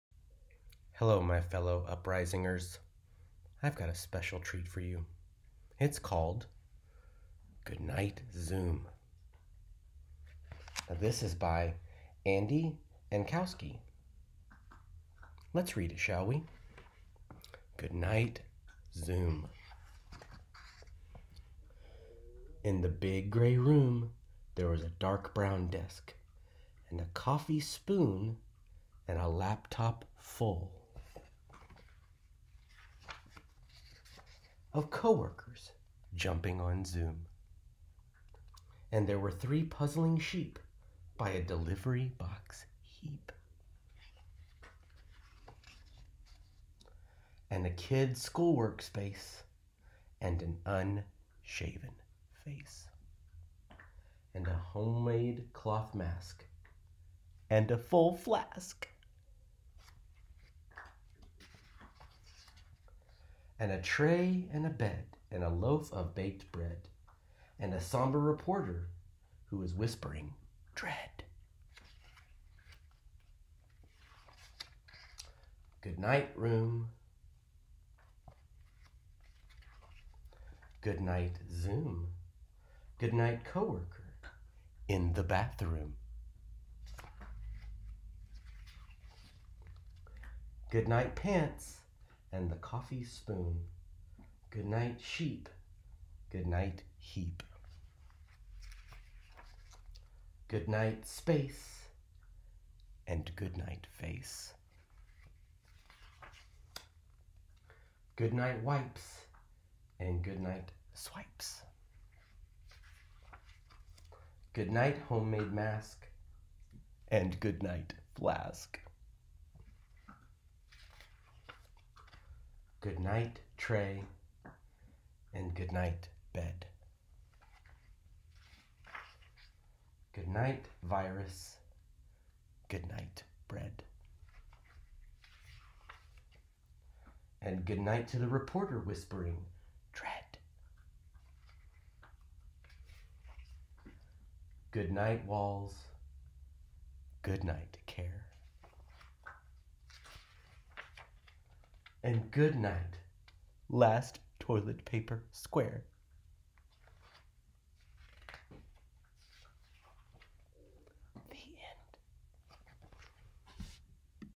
Good Night Zoom
What a perfect way to sum up the year that was. As we say goodnight to 2020, let us read a take on a children's classic. Chris Murman reads to us "Goodnight Zoom" by Andy Ankowski. Enjoy!